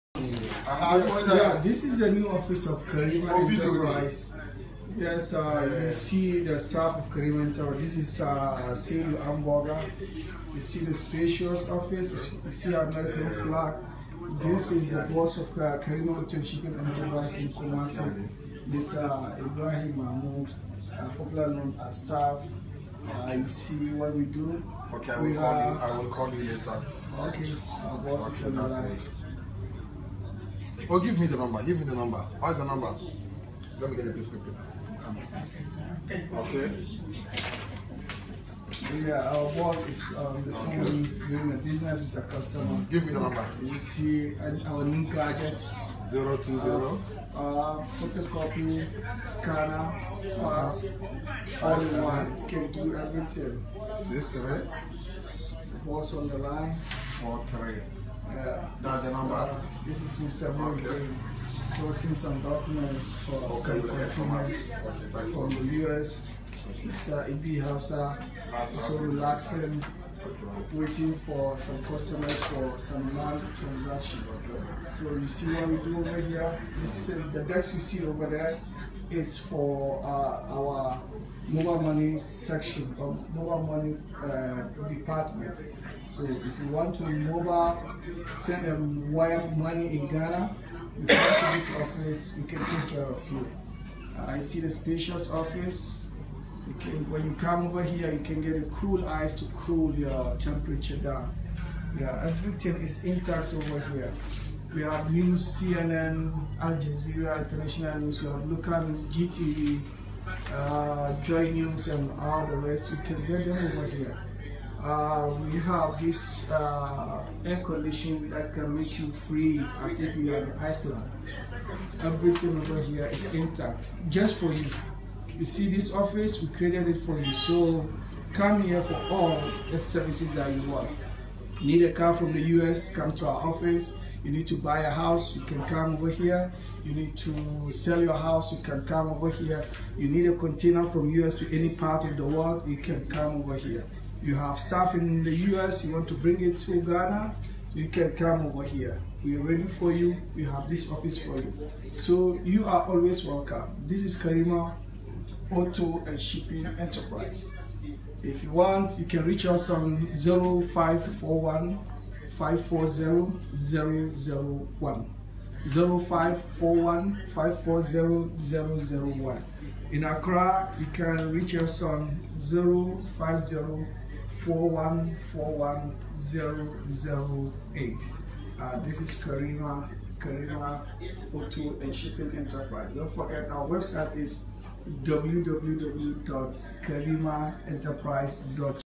New office introduction
Just a brief introduction of our new Kumasi office and staff members. Karima Shipping Enterprise, Safe Delivery of your items is our main priority.